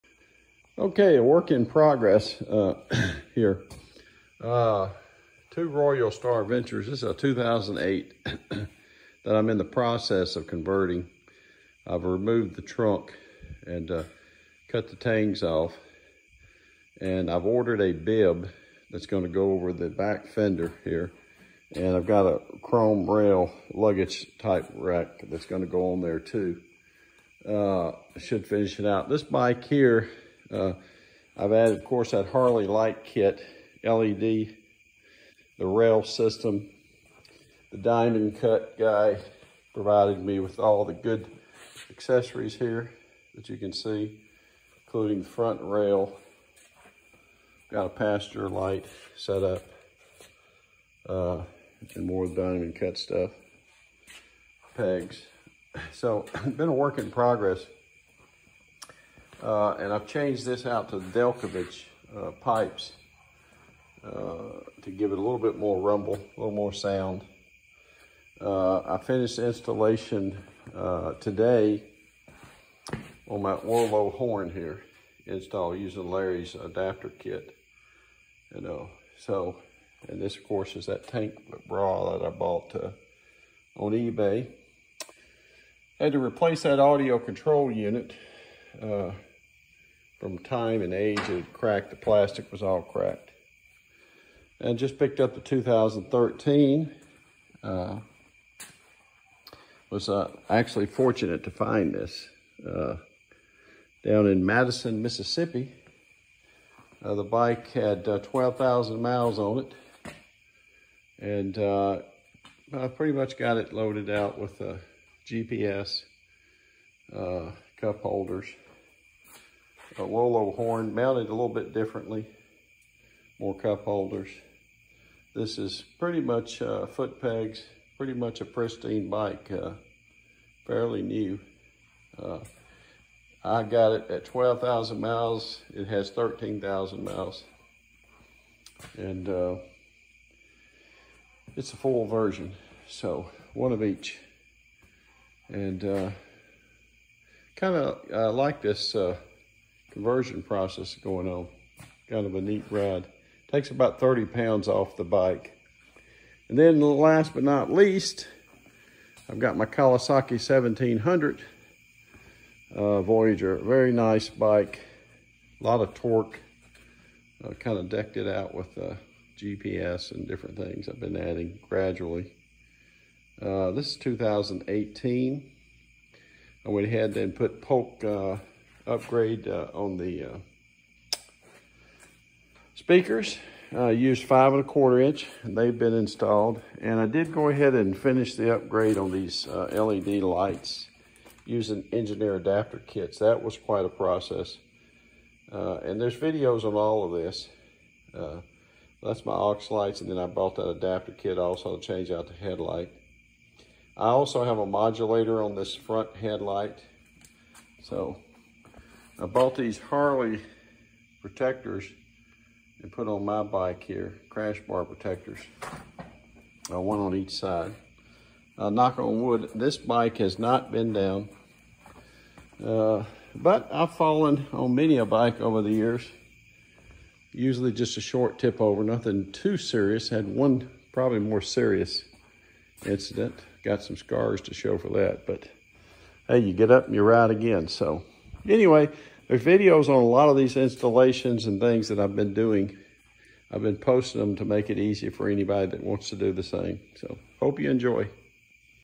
Walk around of various additions Yamaha Royal Star and Kawasaki Vulcan Voyager 1700
I’ve tried to post recently videos to simplify installation of items that we usually add to our bikes.  I have owned all kinds of bikes and wish I
Had more faithfully captured the many rebuilds and simple additions.  Hope you enjoy and maybe find help
If you are looking at adding items